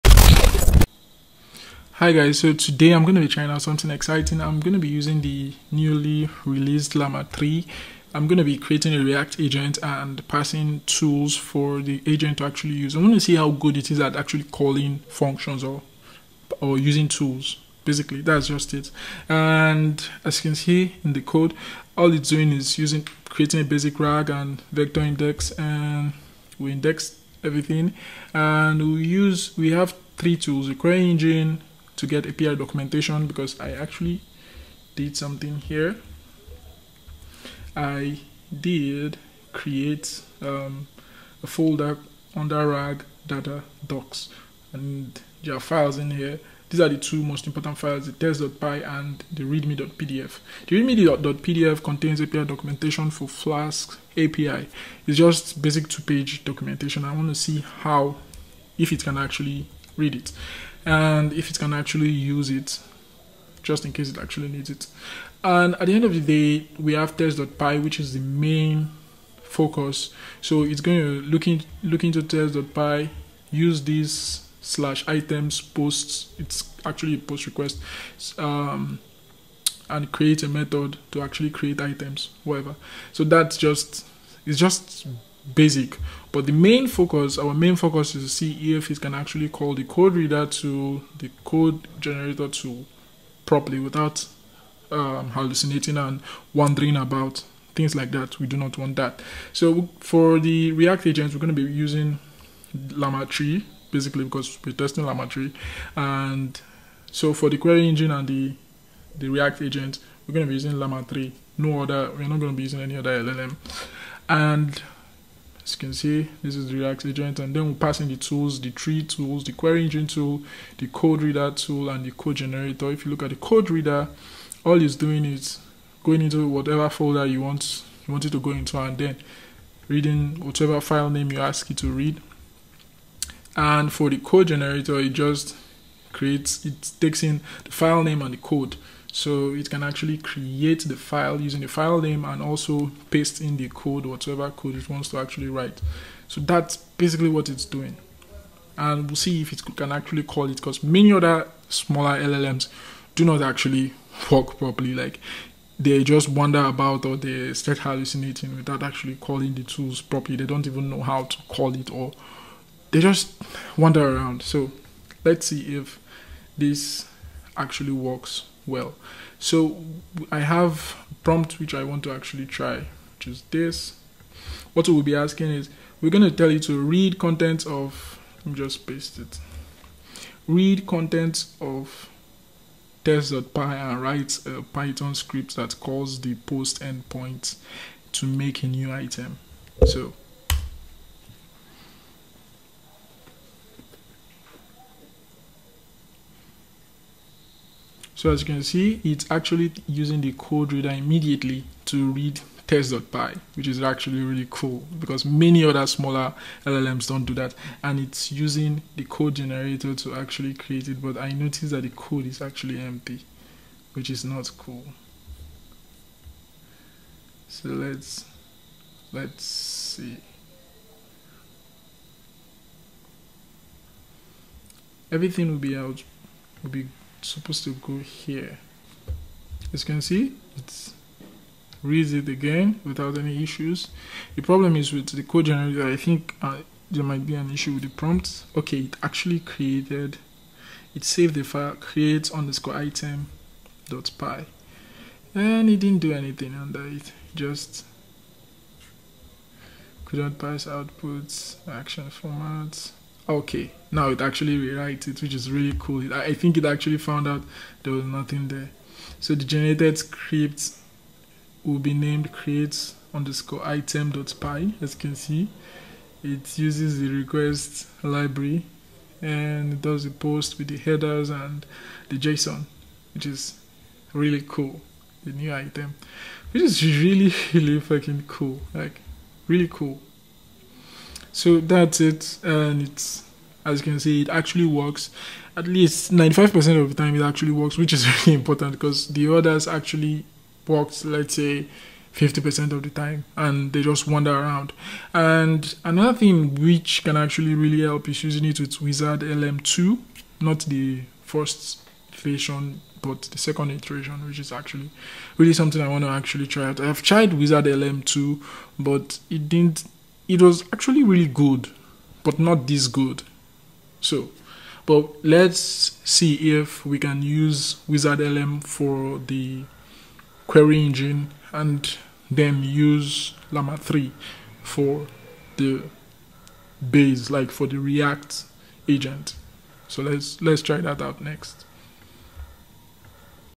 LLAMA 3: function calling review using llama index framework and Ollama locally.
This is a short demo of how well the newly released llama 3 8b performs at function calling. We'll of course, be using the llama index framework to create the ReAct agent and function tools for the LLM.